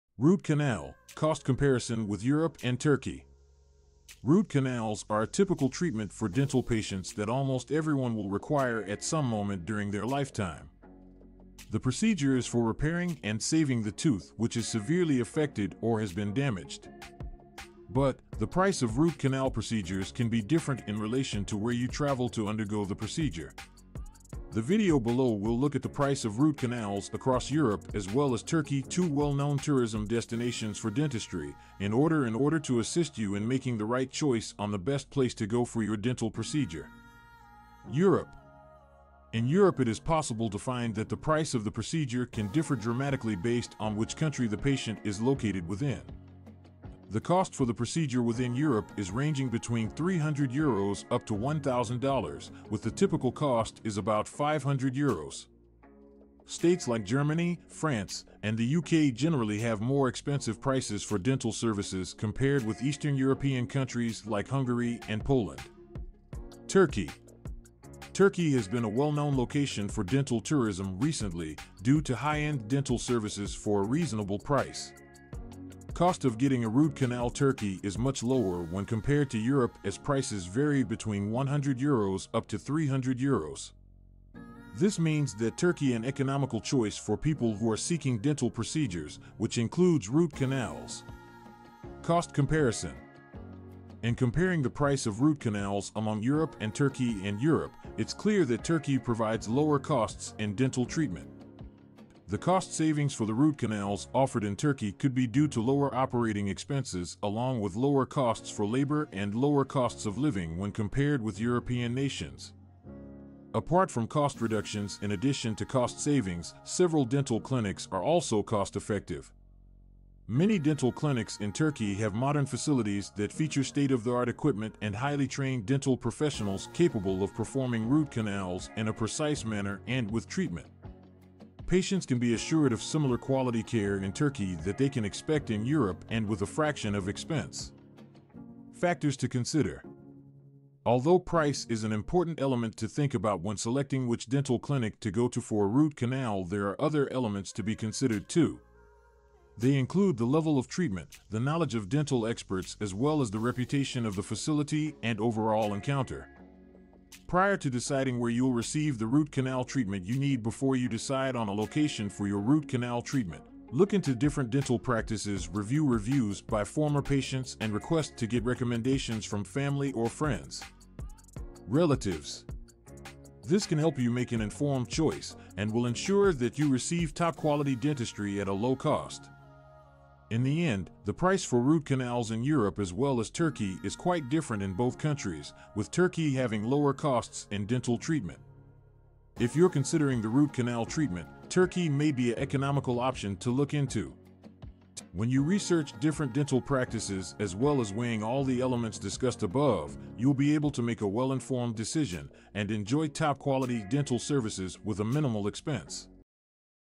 Root Canal Costs in Europe vs Turkey - Which Offers the Best Value for Dental Patients?
Root Canal: Cost Comparison with Europe and Turkey

Root canals are a typical treatment for dental patients that almost everyone will require at some moment during their lifetime. The procedure is for repairing and saving the tooth which is severely affected or has been damaged. But, the price of root canal procedures can be different in relation to where you travel to undergo the procedure. The video below we'll look at the price of root canals across Europe as well as Turkey two well-known tourism destinations for dentistry, in order in order to assist you in making the right choice on the best place to go for your dental procedure. root canal, dental treatment, cost comparison, Europe, Turkey